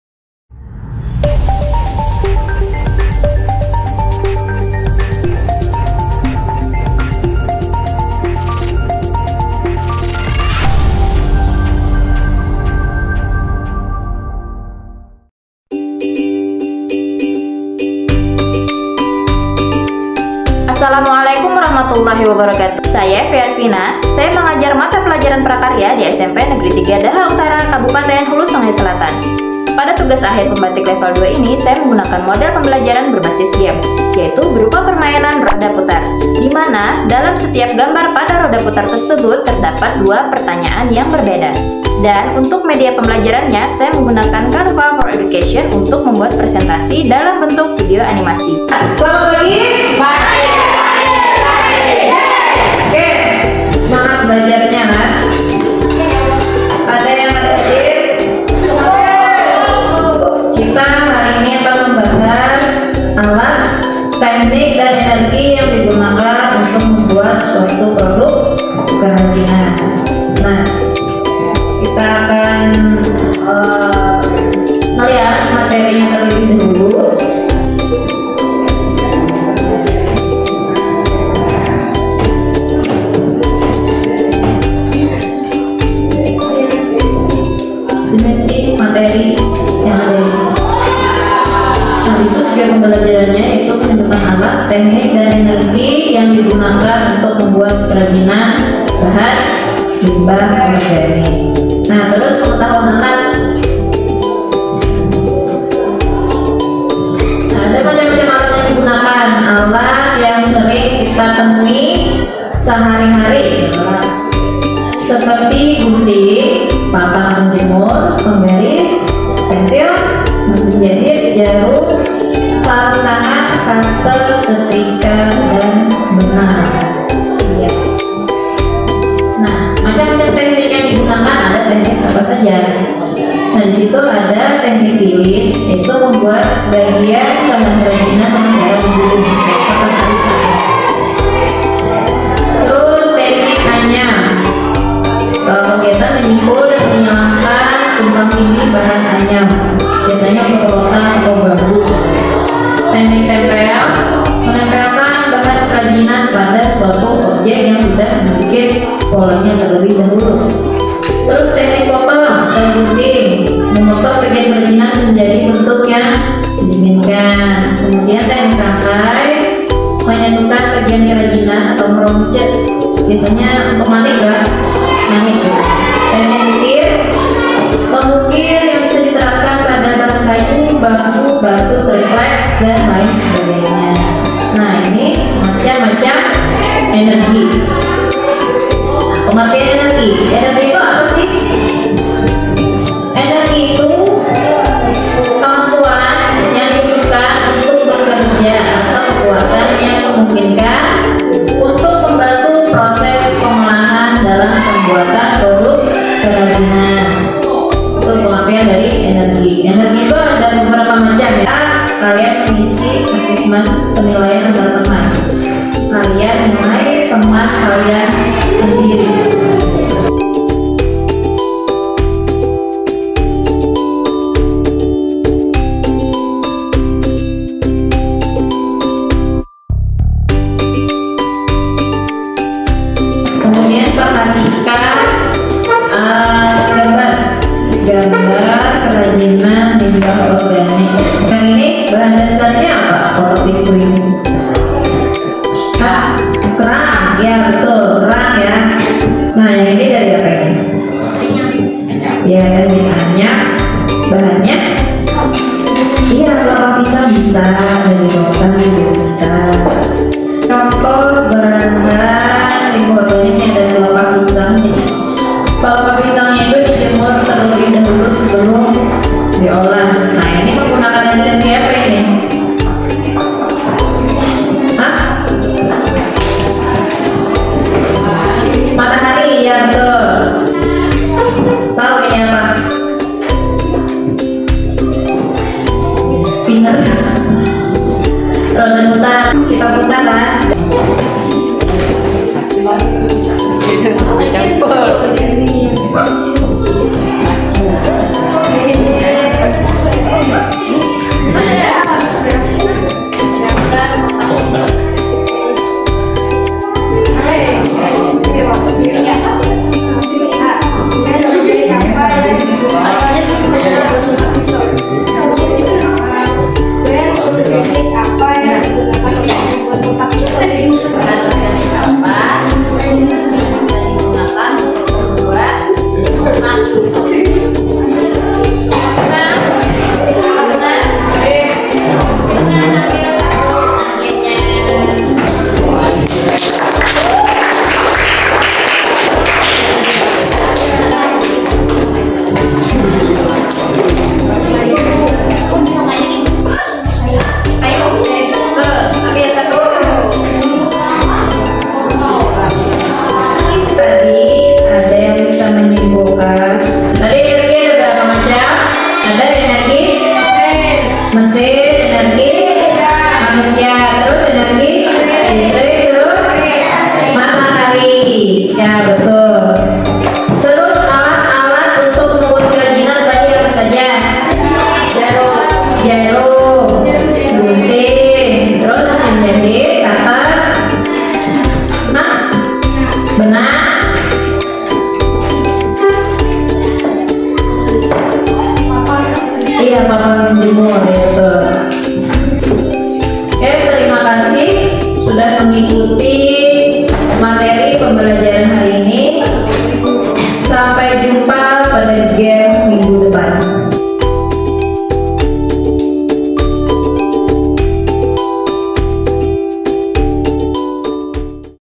Tugas Akhir Pembatik Level 2 2023
Pembelajaran Discovery Learning melalui permainan roda putar.